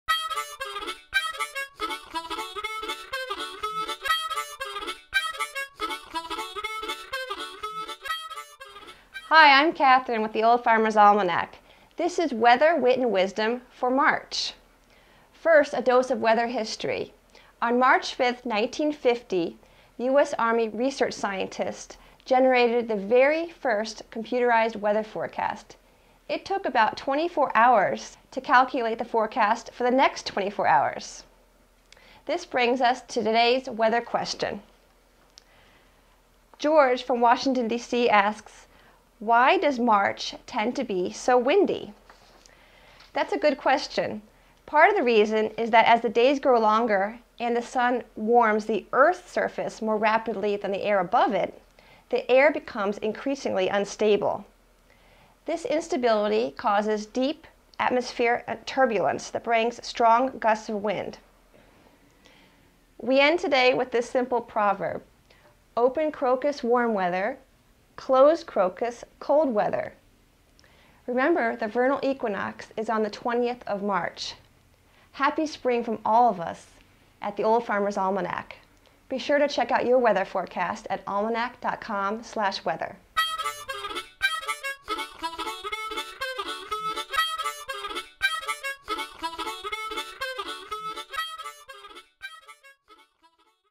Weather Wisdom: Why is March Windy?
Another month of Weather Wit and Wisdom from The Old Farmer's Almanac. This month's question: Why does March tend to be so windy? Find out!